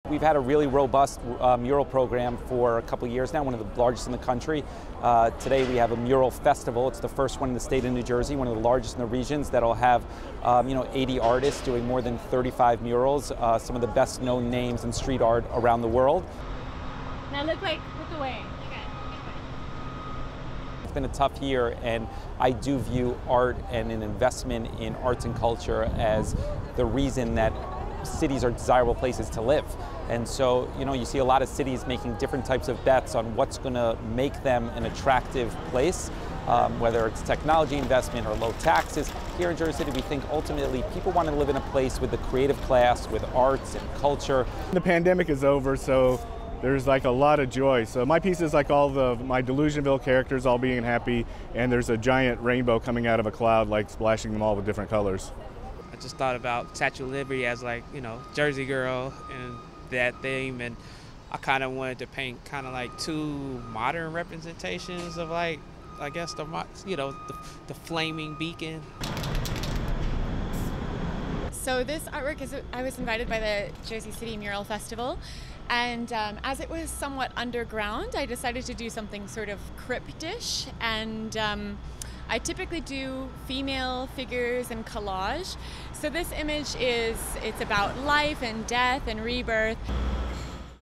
Murals to entice creatives back to cities
More than 50 artists painted murals in Jersey City, New Jersey, on Wednesday, as part of an effort to brand the city as friendly to creatives and bring back people who fled urban areas during the pandemic. (June 2)